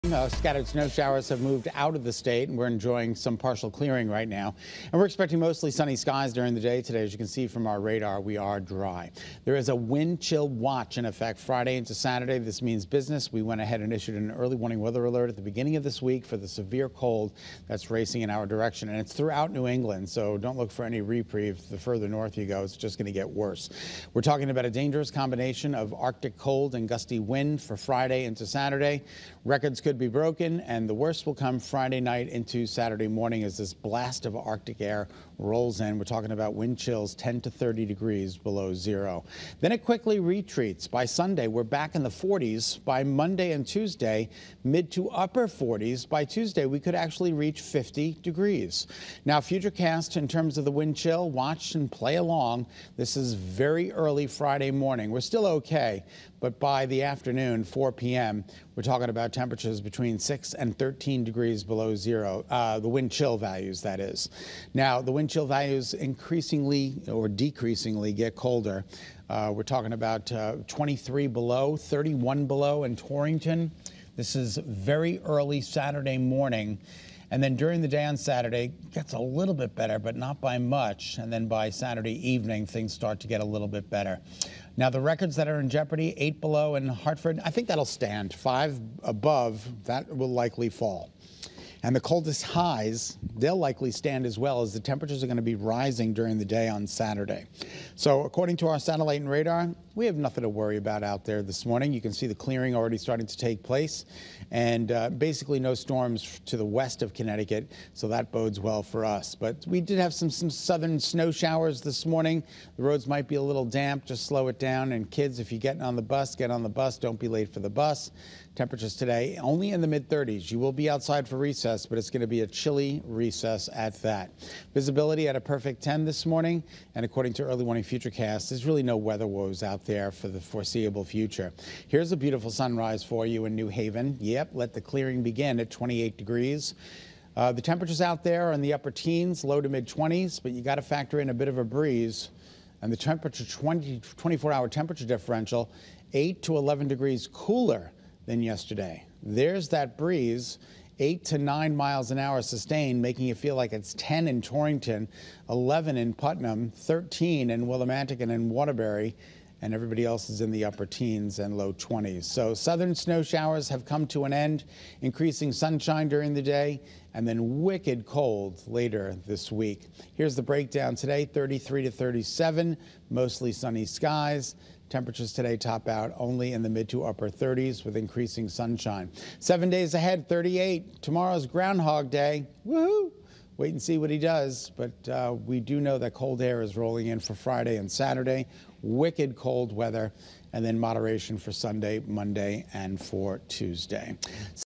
FORECAST: Alert remains on the 7-day for dangerous combo of cold, wind
Meteorologist Scot Haney continued to track an arctic blast that's headed for the state at the end of this week. Here's his Wednesday 7 a.m. forecast.